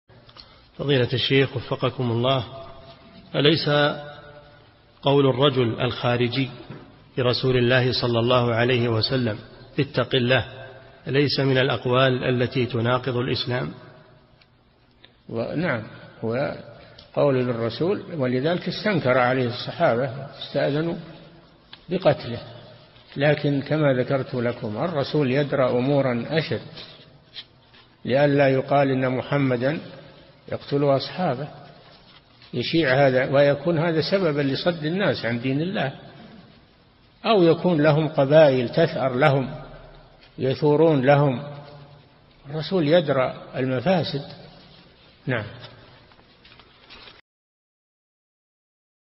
[436 -939] قول الرجل الخارجي لرسول الله ﷺ: اتق الله، هل هي من الأقوال التي تناقض الإسلام؟
-[436 -939] السؤال: أليس قول الرجل الخارجي لرسول الله ﷺ: اتق الله، أليس من الأقوال التي تناقض الإسلام؟
الجواب: نعم، هو قوله للرسول ولذلك استنكر عليه الصحابة واستأذنوا بقتله لكن كما ذكرت لكم الرسول يدرأ أمورًا أشد لئلا يقال أن محمدًا يقتل أصحابه، يشيع هذا ويكون هذا سببًا لصد الناس عن دين الله أو يكون لهم قبائل تثأر لهم يثورون لهم، فالرسول يدرأ المفاسد.
للاشتراك في قناة الشيخ صالح الفوزان

للاستفسار والإقتراحات والدعم
رئيس الفريق
لمشروع كبار العلماء